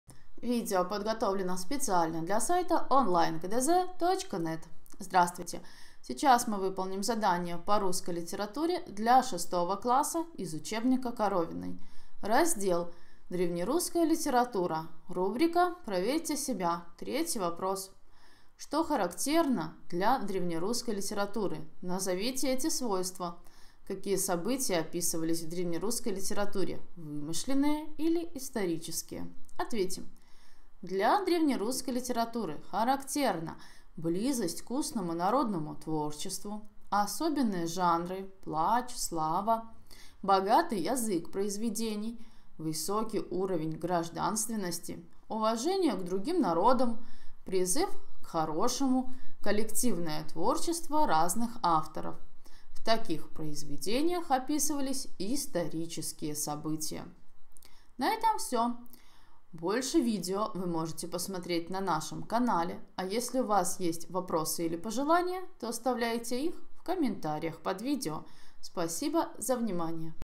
Вопрос №3 Древнерусская литература. Проверьте себя — Литература 6 класс (Коровина В.Я.) Часть 1
В 3 задании из рубрики «Проверьте себя» нужно указать, что характерно для древнерусской литературы, назвать эти свойства, определить, какие события описывались в древнерусской литературе (вымышленные или исторические). В помощь вам – наше видео с решением!